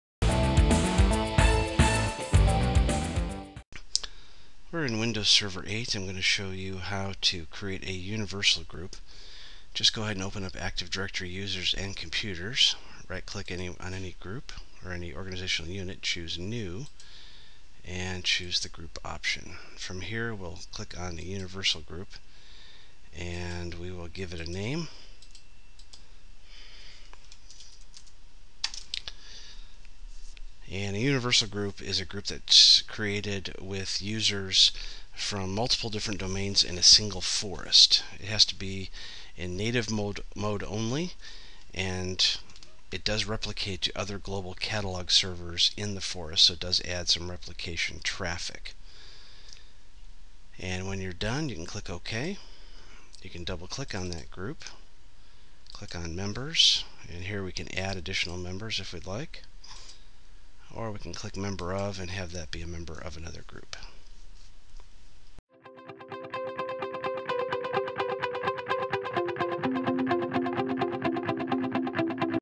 How to create a Universal Security Group in Microsoft Windows Server 2012
Author and talk show host Robert McMillen explains how and why to create a Universal Security Group in Microsoft Windows Server 2012